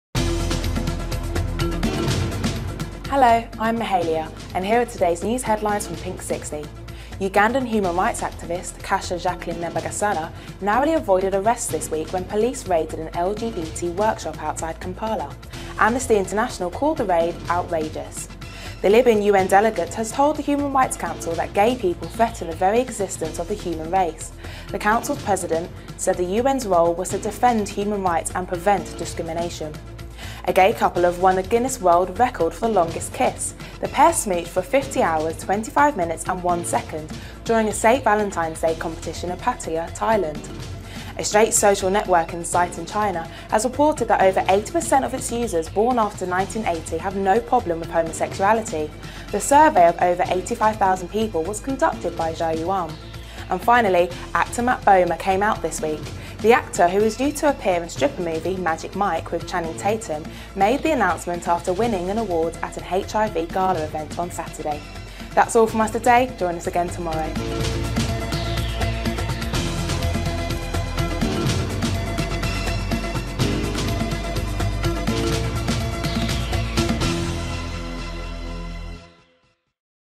Pinksixty News WEDNESDAY 15 FEBRUARY 2012
Ugandan Human Rights activist, Kasha Jacqueline Nabagasera narrowly avoided arrest this week when police raided an LGBT workshop outside Kampala. Amnesty International called the raid 'outrageous'.

The Libyan UN delegate has told the Human Rights Council that gay people threaten the very existence of the human race. The Council President said the UN's role was to "defend human rights and prevent discrimination."

A gay couple have won the Guinness World Record for the longest kiss. The pair smooched for 50 hours, 25 minutes and 1 second during a St Valentine's competition in Pattaya, Thailand.

A straight social networking site in China has reported that over 80% of its users born after 1980 have no problem with homosexuality. The survey of over 85,000 people was conducted by Jiayuan.

And finally, actor, Matt Bomer came out this week. The actor, who is due to appear in stripper movie 'Magic Mike' with Channing Tatum, made the announcement after winning an award at an HIV gala on Saturday.